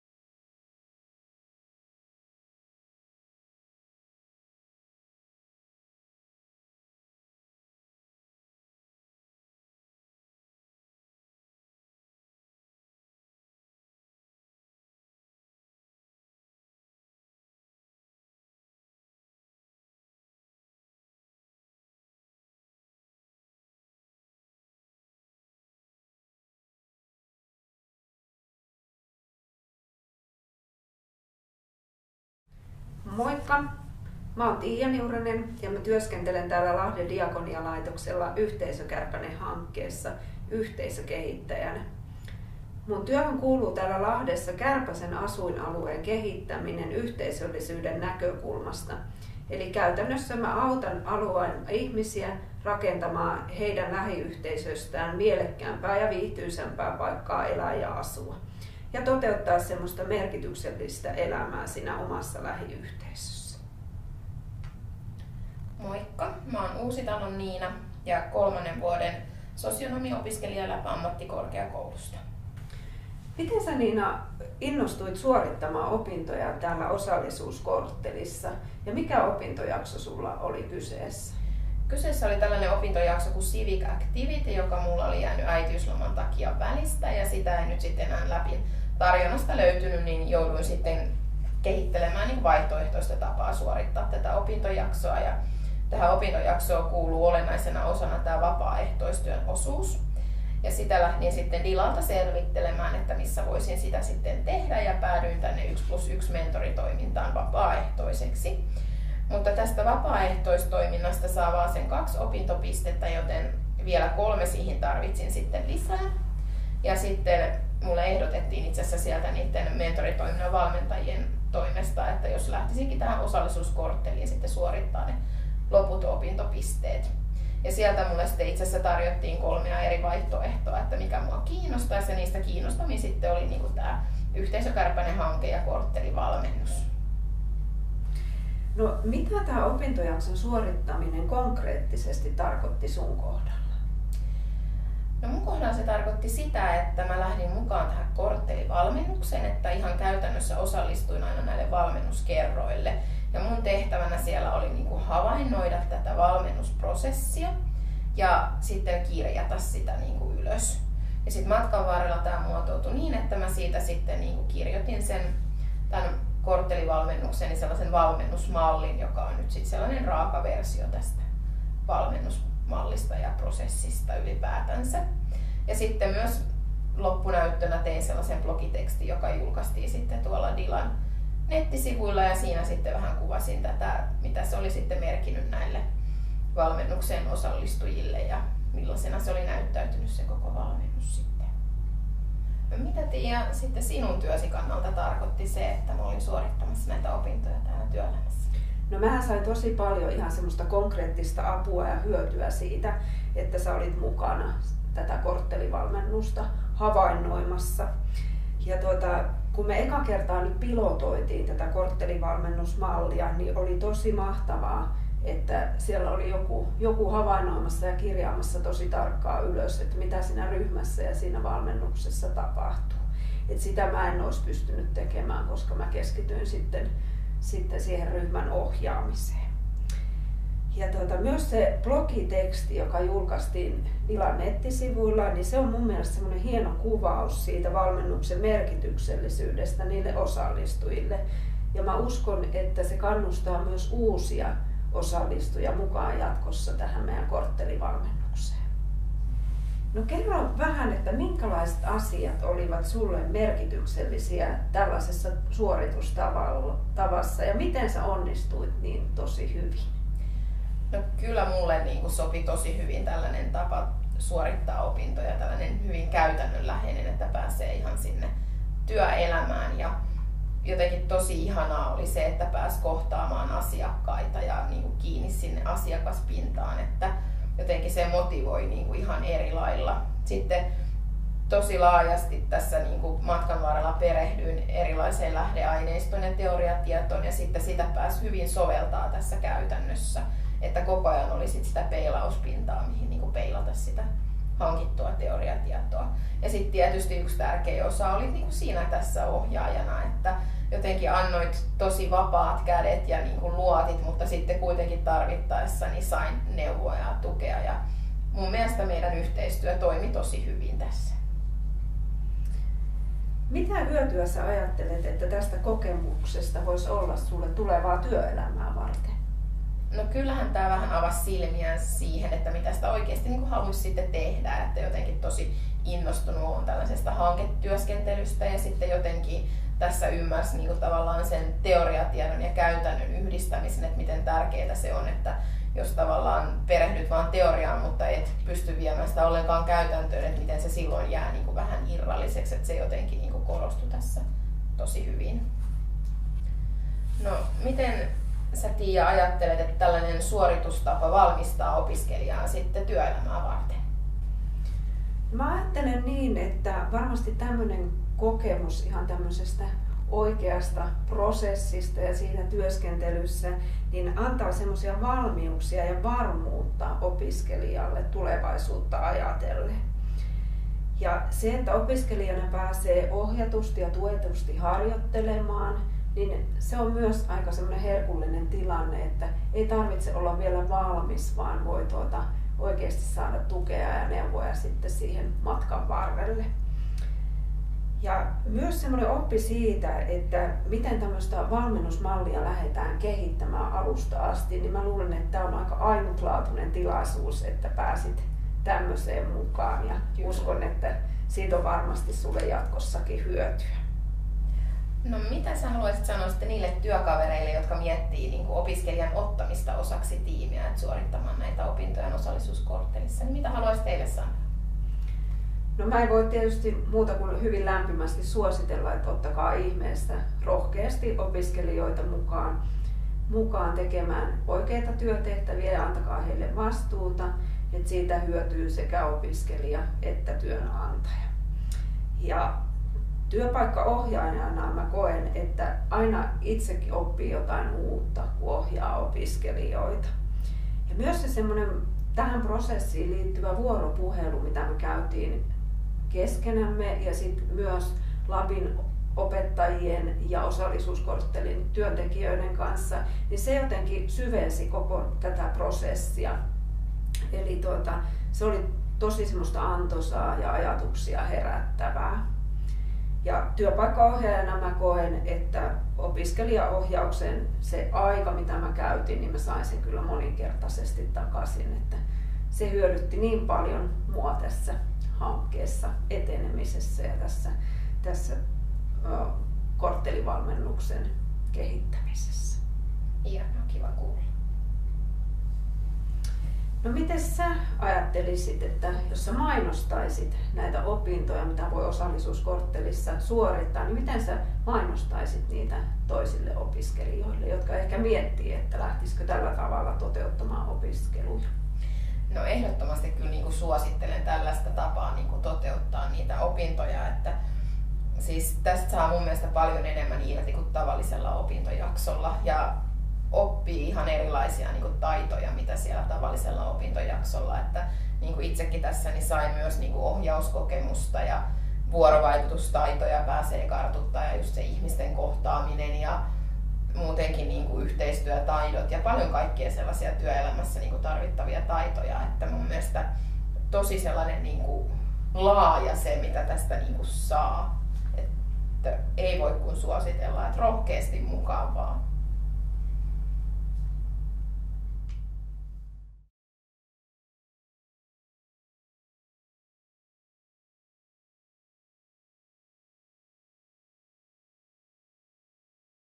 Omannäköistä oppimista Osallisuuskorttelissa
Osallisuuskortteli-hanke mahdollistaa LABin sosionomiopiskelijoille uudenlaisia tapoja suorittaa puuttuvia opintoja aidoissa työympäristöissä Lahden diakonialaitoksen Dilakorttelissa tai Harjulan Setlementin toiminnoissa.
Käsikirjoitus ja toteutus: Osallisuuskortteli-hanke; Tiia Kangassalo, Lahden diakonialaitos; Virve Pirttikoski ja Tarja Kempe-Hakkarainen, LAB-ammattikorkeakoulu
Kuvaus ja leikkaus: Elias Kosonen, Lahden diakonialaitos
Videolla keskustelevat: yhteisökehittäjä Tiia Niuranen, Lahden diakonialaitos, ja sosionomiopiskelija Nina Uusitalo, LAB-ammattikorkeakoulu.